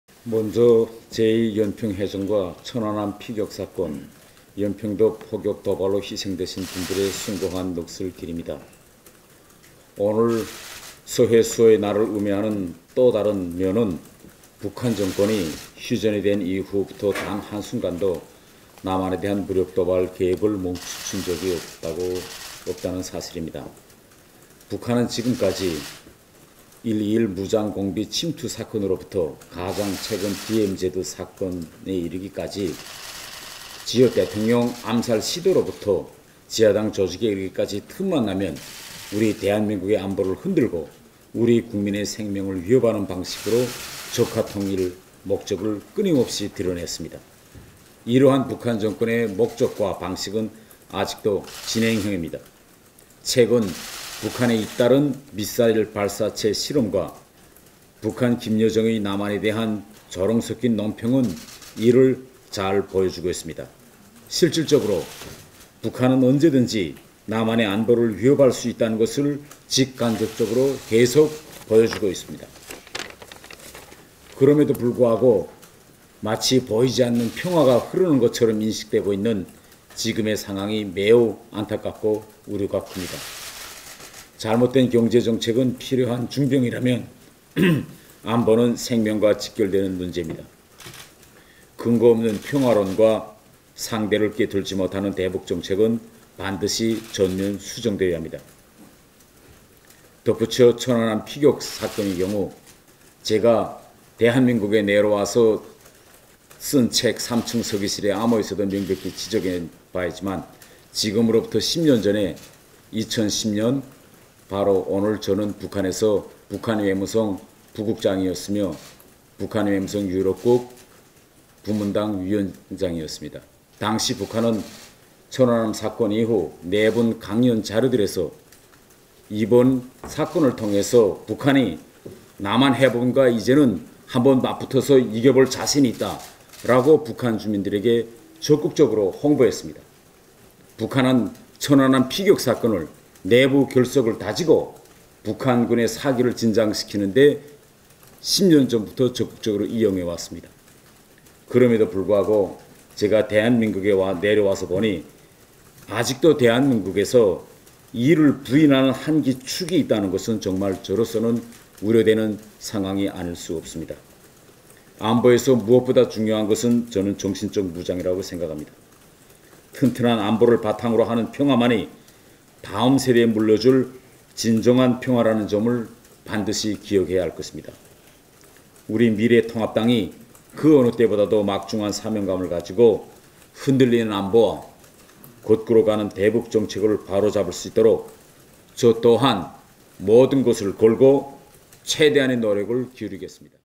태구민 "상대를 꿰뚫지 못하는 대북 정책 전면 수정되어야 한다"
태구민 미래통합당 서울 강남구갑 후보가 27일 국회에서 열린 선거대책위원회 전략회의에서 대북정책과 관련해 발언하고 있다.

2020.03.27 일요서울TV 권민구 기자